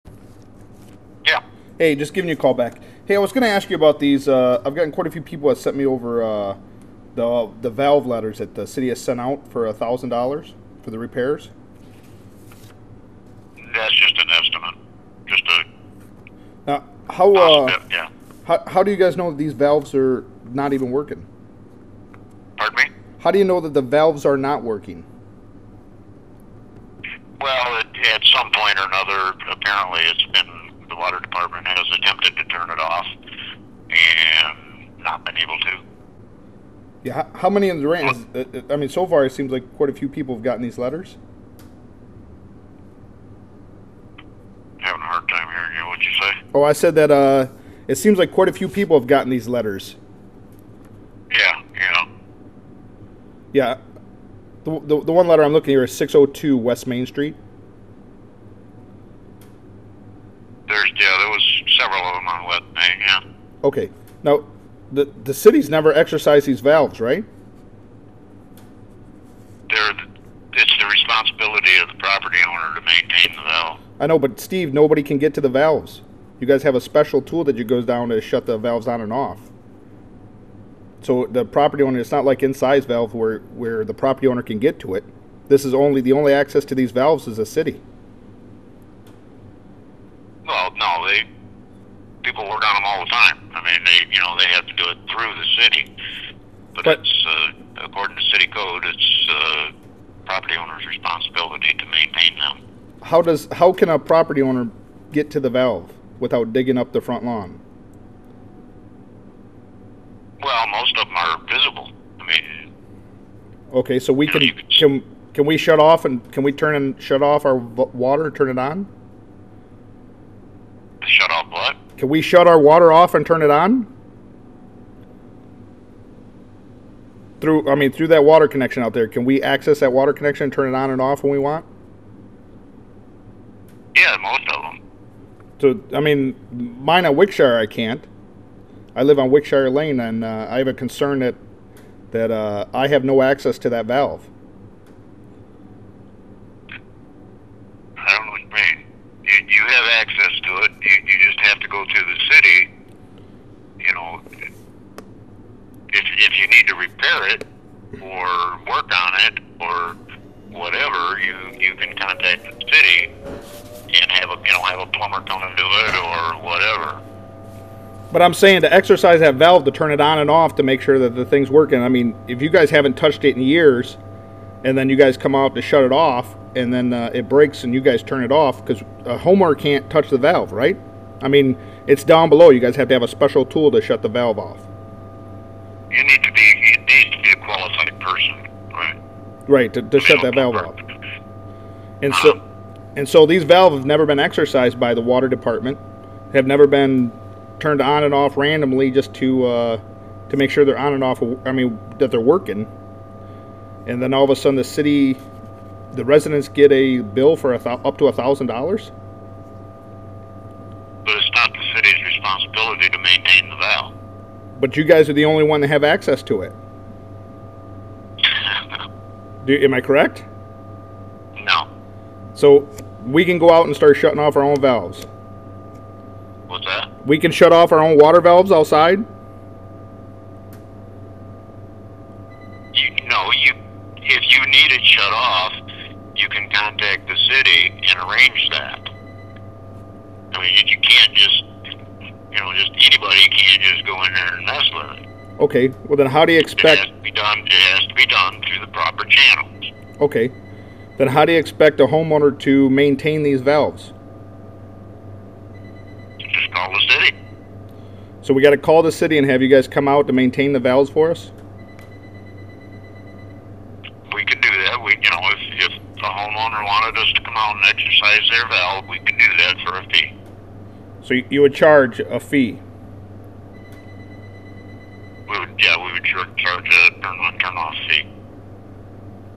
City Water valves Steve Mince Durand Dept Public Works Durand Michigan
Follow up on water valve issues:
A letter was sent to a homeowner telling them they had to replace a Water valve or the city would charge them up to $1000 to do it.

Follow up from the homeowner:
Yes it shuts off the city came out and I made them show me Turns out they were wrong and then blamed it on the computer ... either way my valve works great

Change in Durand: How many other people got this letter, repaired the Water valve when they shouldn't have.